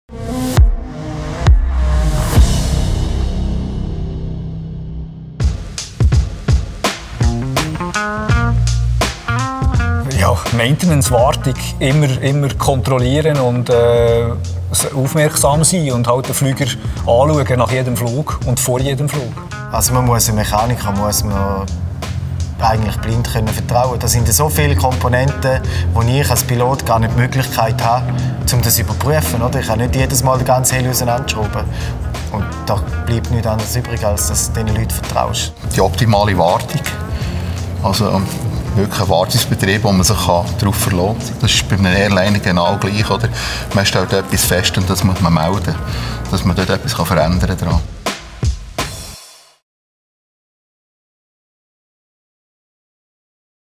Ask a pilot | What makes a flight instrument reliable?
ÜBER DIE AVIATORS
Umgeben von ihren Flugzeugen, dem Geruch von Kerosin und dem Lärm des Flughafenalltags treffen sich unsere drei verschiedenen Piloten zu einem Gespräch:

- Marc Rollier, Turbine-Legend Pilot
- Silvio Ludin, Hubschrauberpilot
- Rolf Bläsi, Oldtimer-Pilot

ASK A PILOT
soll die Fragen beantworten, die wir alle schon mal einem echten Piloten stellen wollten.

IN DIESEM VIDEO:
Wir wollten wissen, was die Fluginstrumente zuverlässig macht. Sieh dir an, was die AVIATORS dazu meinen...